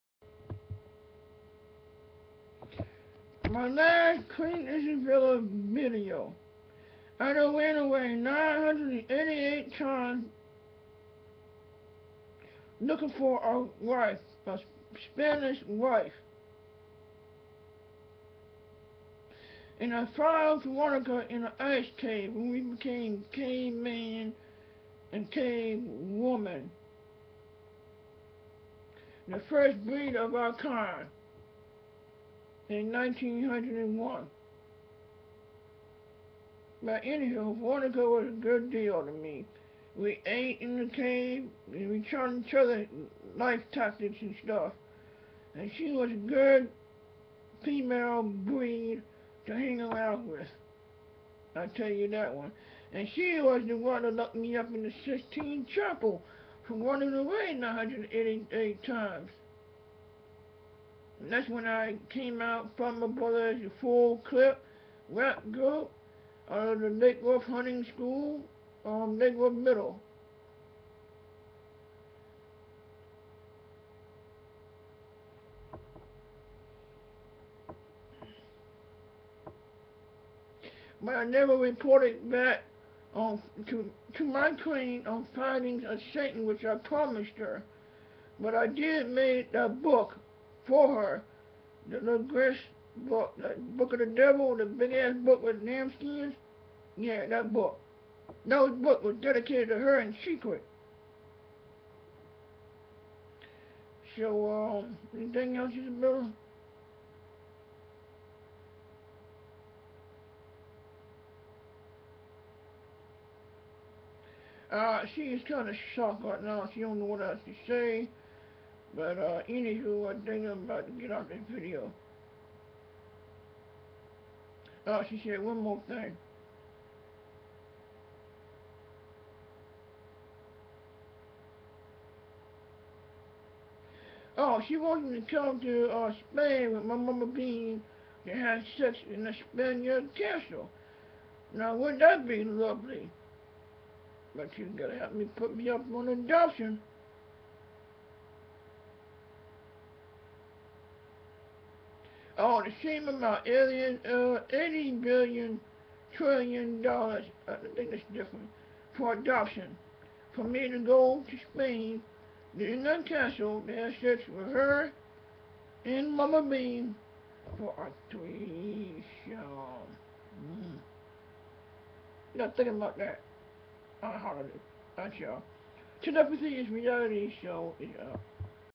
Last Isabella Telepathy is relativity show
She tells me in Telepathy that i must marry the Queen, for a year.